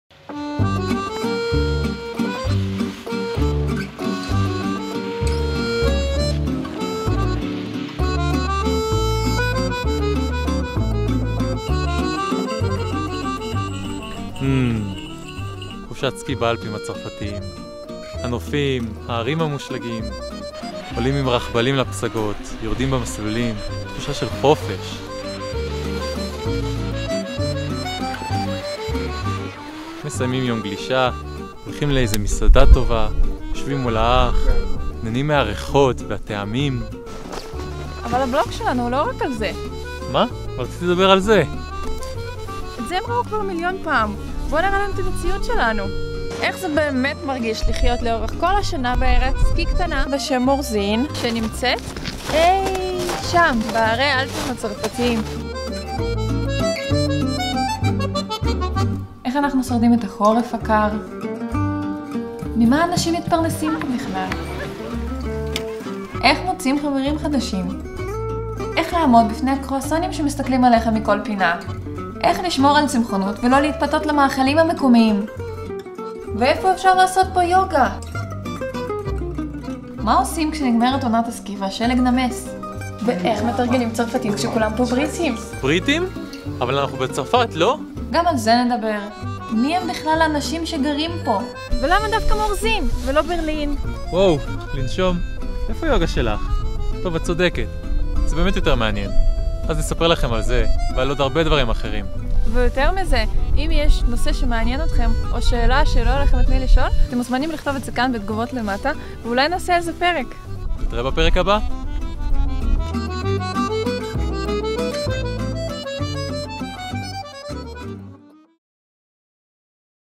Living in the Alps - Dream VS Reality
Want to see how it’s really like to live all-year-around in Morzine, a ski resort in the French Alps?
On our vlog we'll show you how we overcome the daily life challenges with yoga and mindfulness. We're Reuben & Polly, a couple of photographers who moved from a city to a small town of Morzine, French Alps. We're going to show you how our real life here in the mountains looks like.